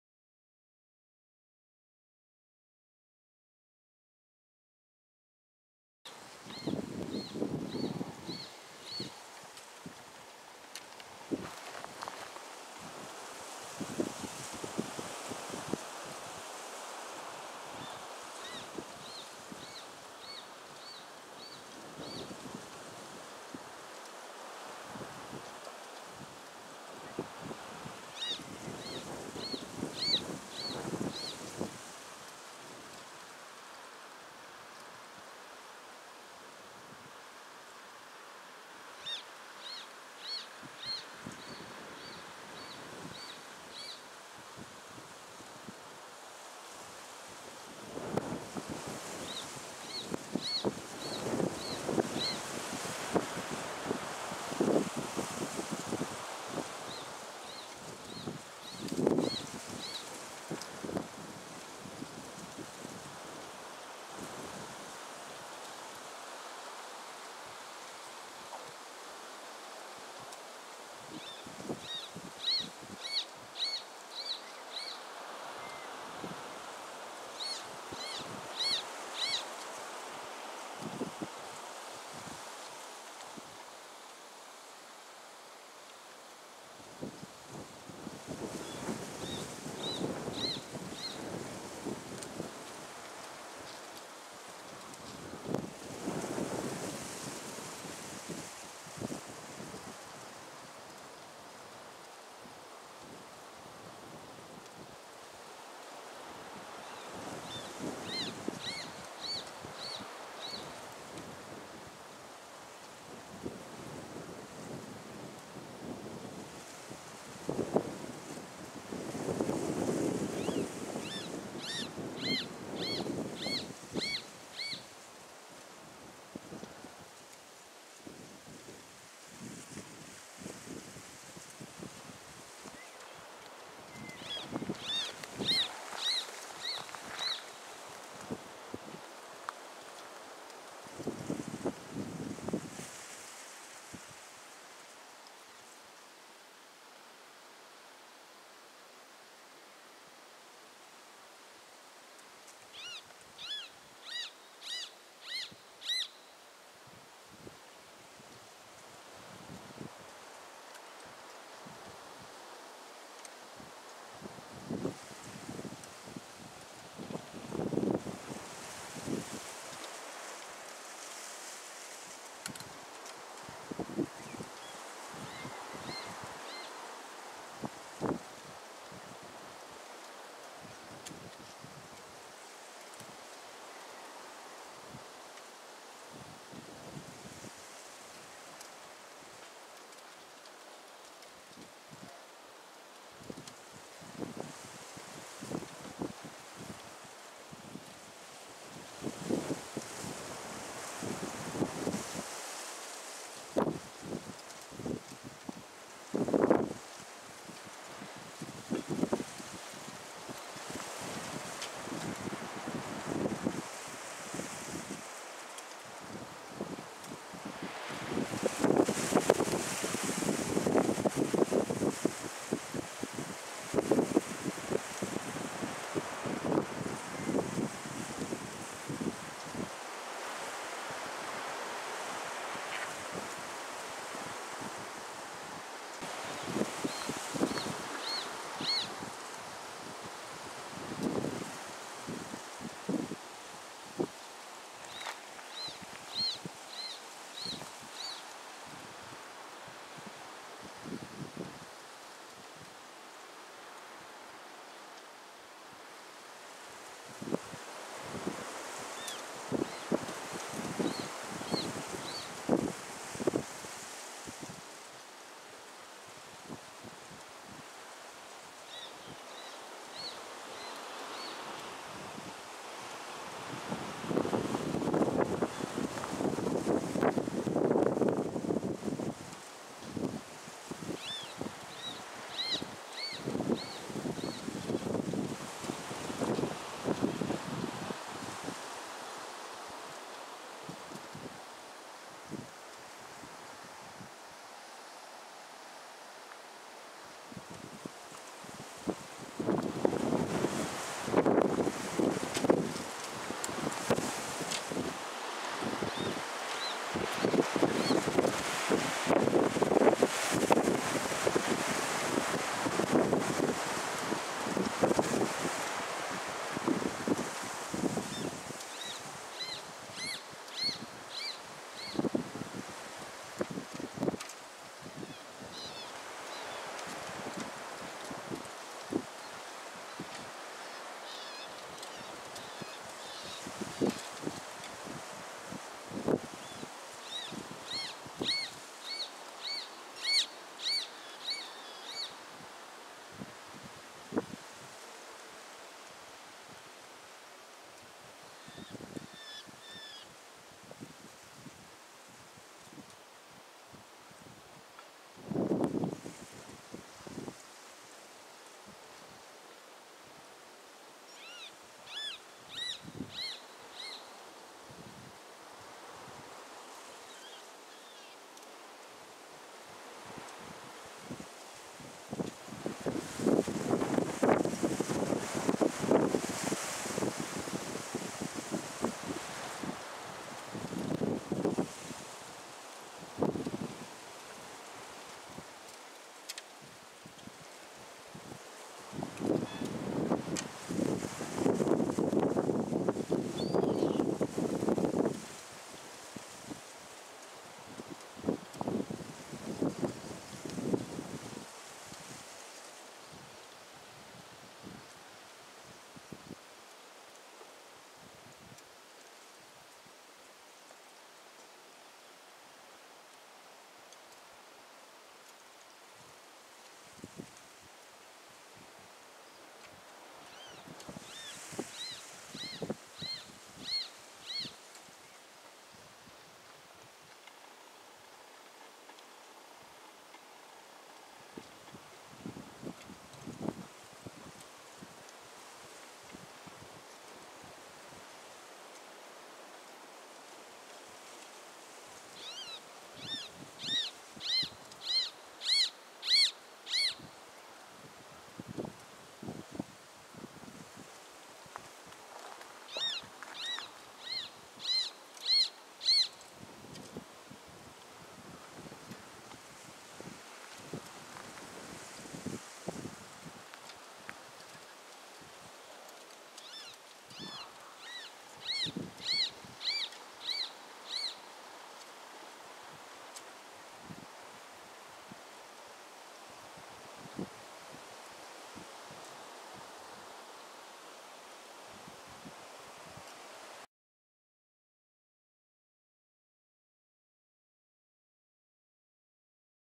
Canto do Gavião Carijó - Pássaro do Sertão
O piado do Gavião Carijó, uma ave do bioma Caatinga, Sertão.